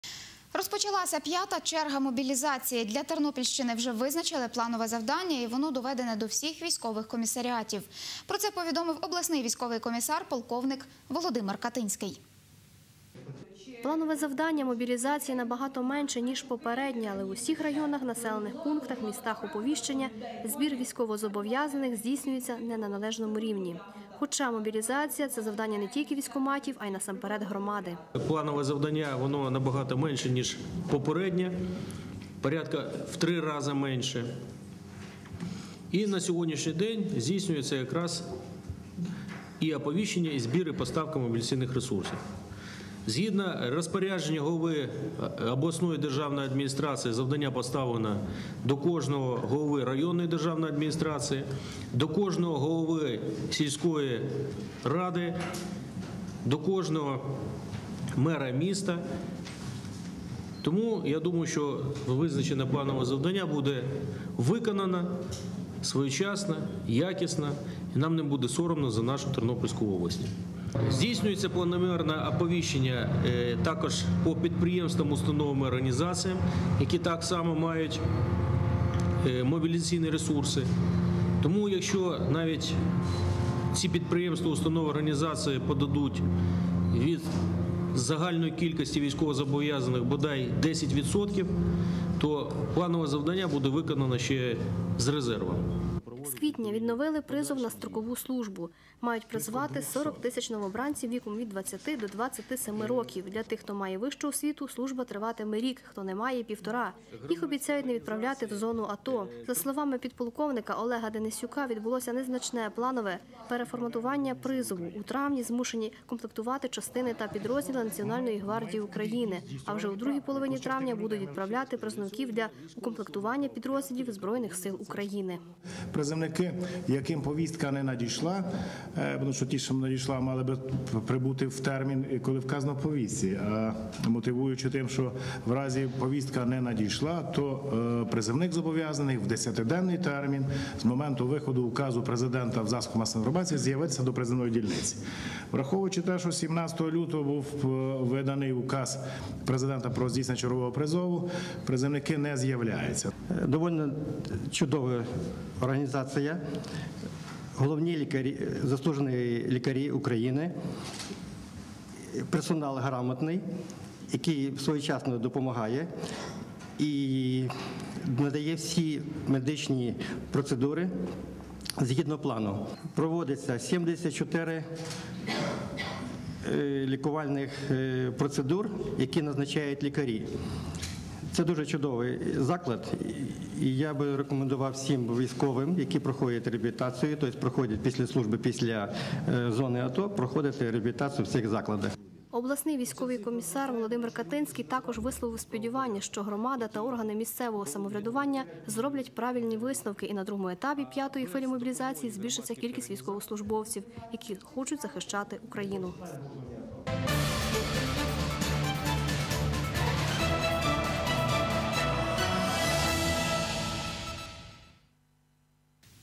На Тернопіллі розпочалася п'ята черга мобілізації.
Розпочалася п'ята черга мобілізації. Для Тернопільщини вже визначили планове завдання, і воно доведене до всіх військових комісаріатів. Про це повідомив обласний військовий комісар, полковник Володимир Катинський.